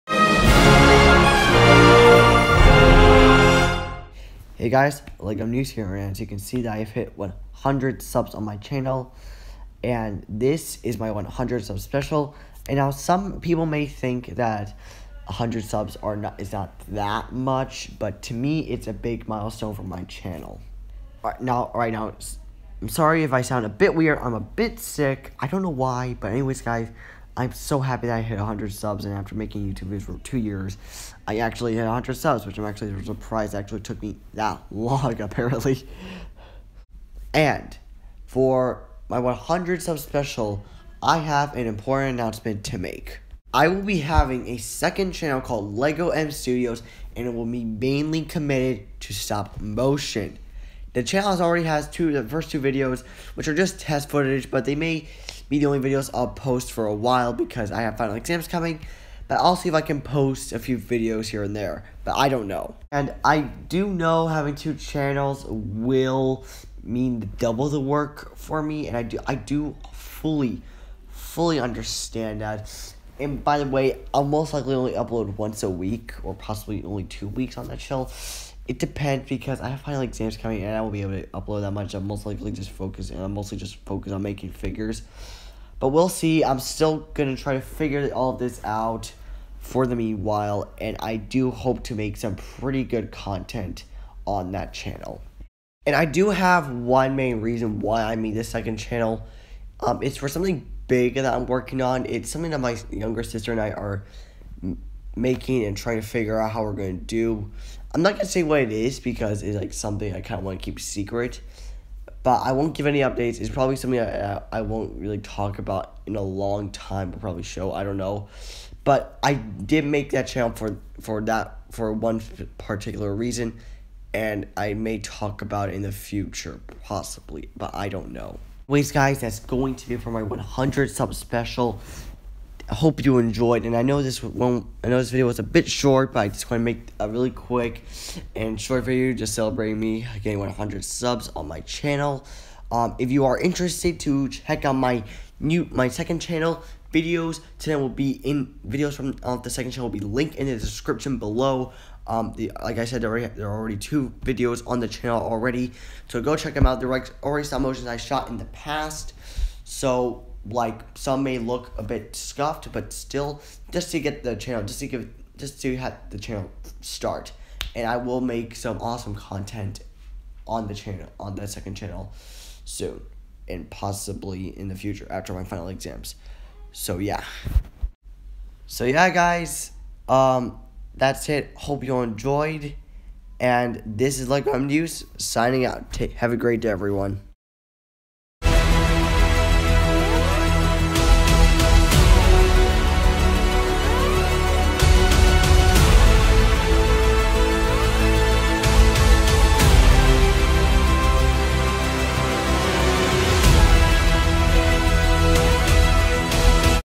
My 100 Sub Special and Announcement!
This is my 100 sub Special, I have been making videos for more than 2 years now and I am surprised that it took me that long to get a 100 subs. Some people may think its not that much but its a huge step for the future of my channel.

And I made this video to announce my second channel called Legomstudios  and if you want to check out my second channel than the links to the first videos are right here:

This is Legomnews signing out.